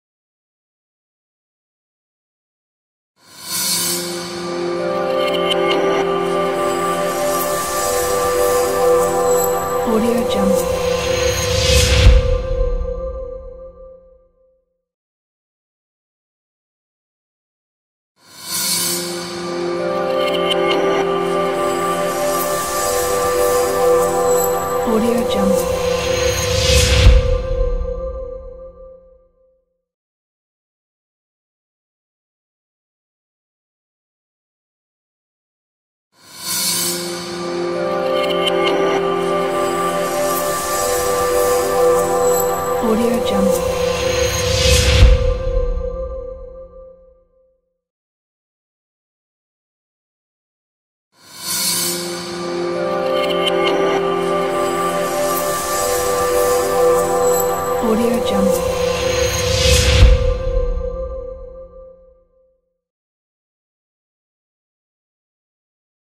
Digital Code Intro Video Effects & Stock Videos from VideoHive
A modern, hi-tech, complex digital code logo reveal with customizable particles for a business presentation, website, image video or media channel.

Stardust (vs 0.9.5 or higher) plugin is REQUIRED to use this template, Get it

If you don´t have Stardust or After Effects, I do offer customization services for my templates!

Template Features:
    * Works with logos and text layers
    * Two versions included (dark & blueprint)
    * Easy to customize – everything is customizable, no pre-renders
    * Code particle texture is editable
    * Step by step customization VIDEO TUTORIAL included
    * Audio is not included in the download – you can get it

Technical Details:
    * UltraHD (4K) and FullHD resolution
    * Compatibility: After Effects CC2015 and above
    * Stardust (v0.9 or higher) plugin REQUIRED